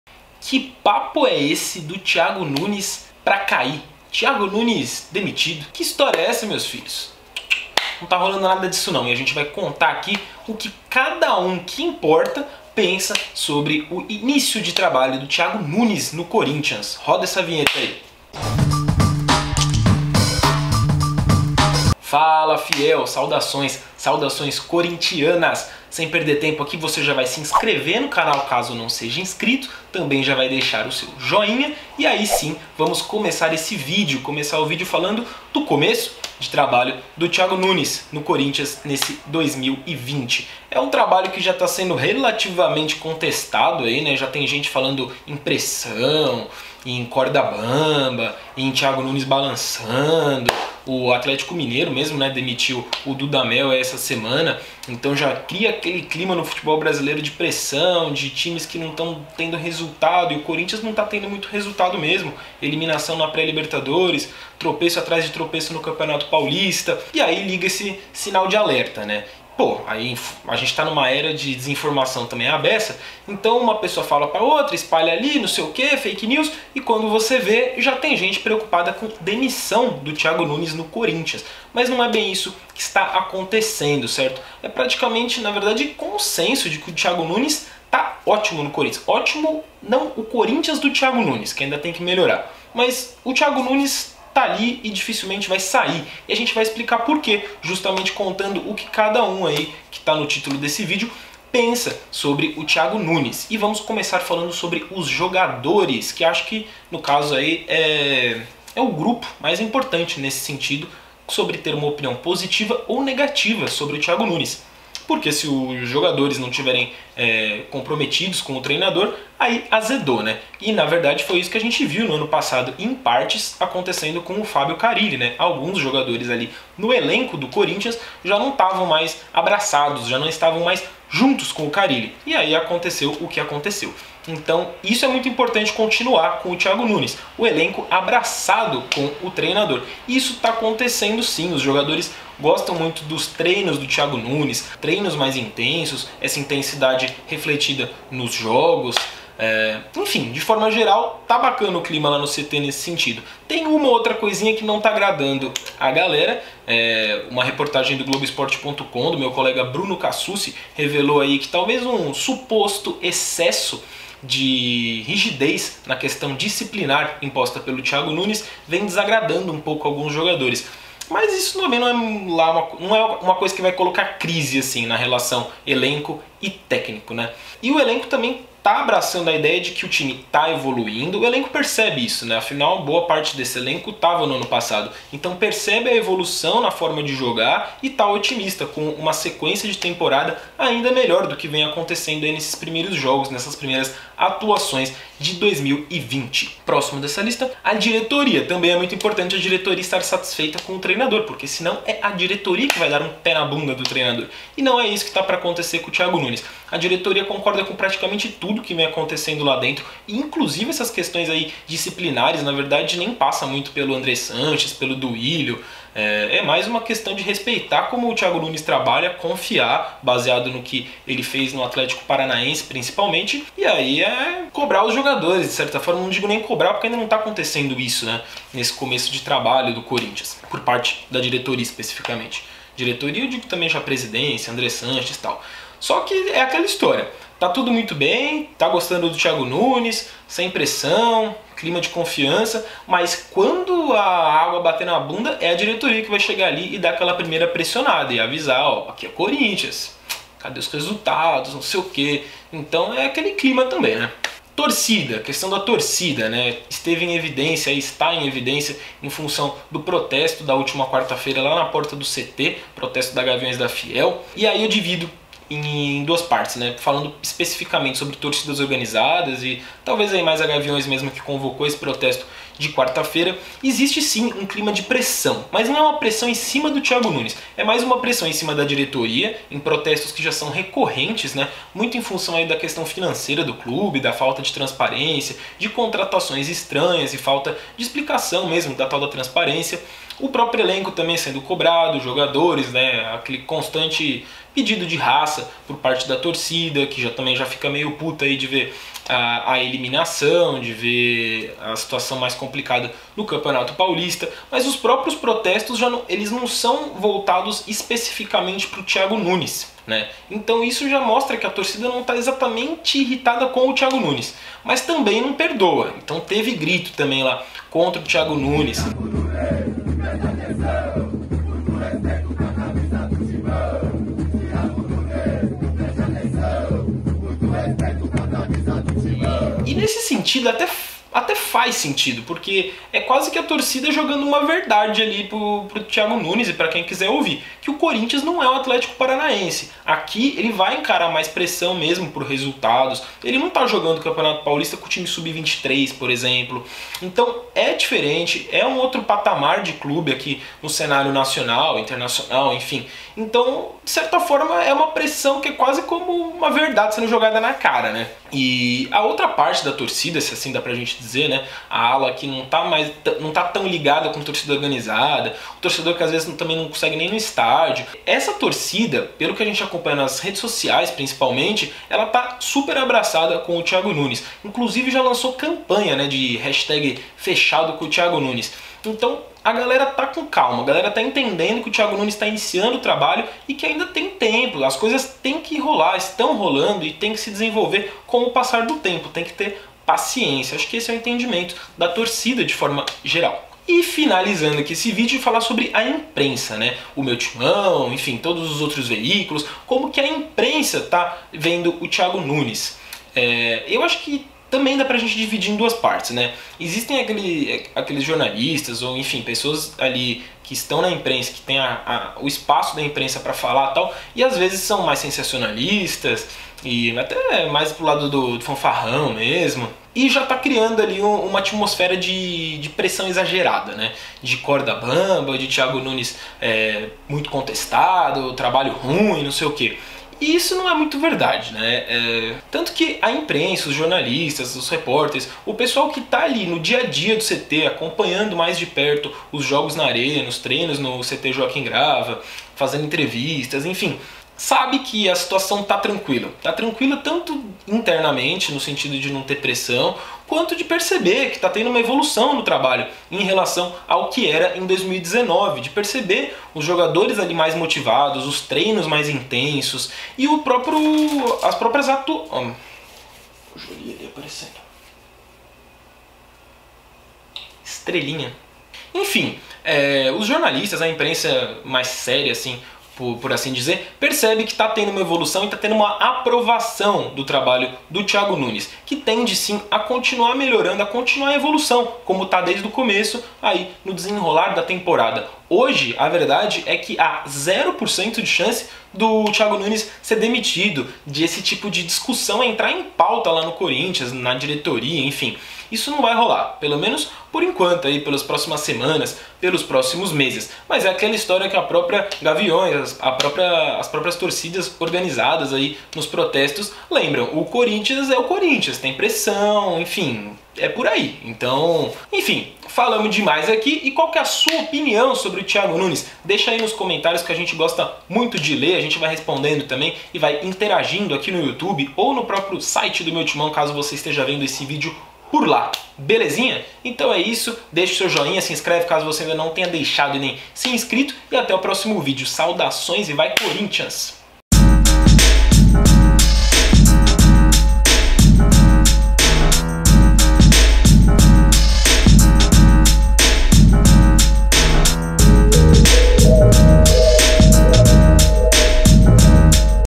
O QUE DIRETORIA, JOGADORES, IMPRENSA E TORCIDA PENSAM SOBRE TIAGO NUNES
Tiago Nunes começou a ser contestado no Corinthians? Não é bem assim... O Meu Timão mostra o que cada um pensa sobre o treinador nesse início de 2020.  E cada caso é um caso... Jogadores, dirigentes, torcida e imprensa!

Técnico do Corinthians se coloca no lugar da torcida em momento difícil: 'Eu estaria insatisfeito'